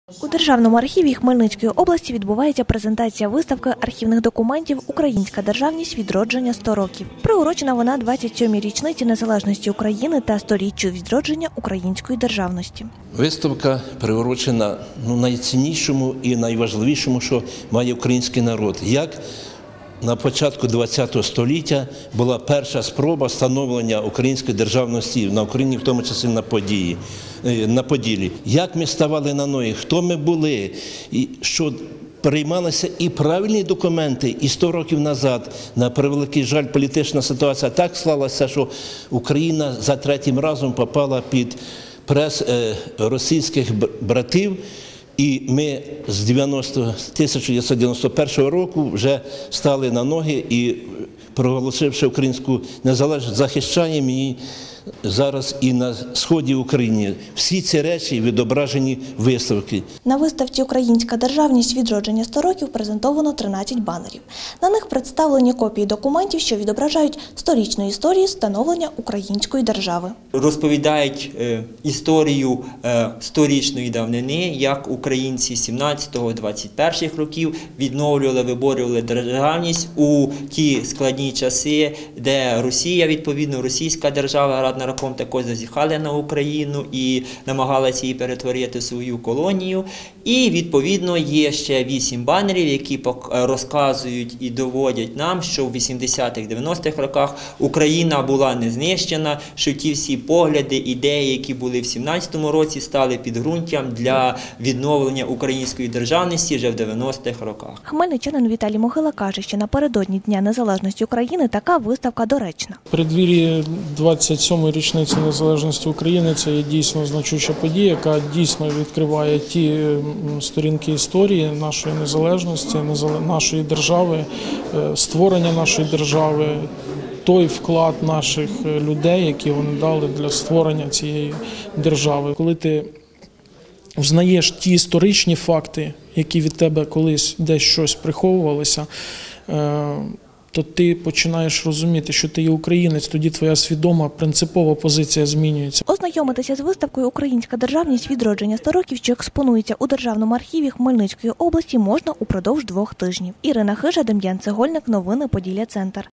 Презентація виставки архівних документів відбулася у Хмельницькому
Сьогодні відбулося відкриття виставки архівних документів «Українська державність: Відродження 100 років» у Державному архіві Хмельницької області. Хмельницький став першим містом, де була представлена ця експозиція з нагоди 27-ї річниці незалежності  та 100-річчя відродження української державності. На презентованих банерах розміщені копії архівних документів. Вони були створені упродовж сторіччя, починаючи з 1917 року.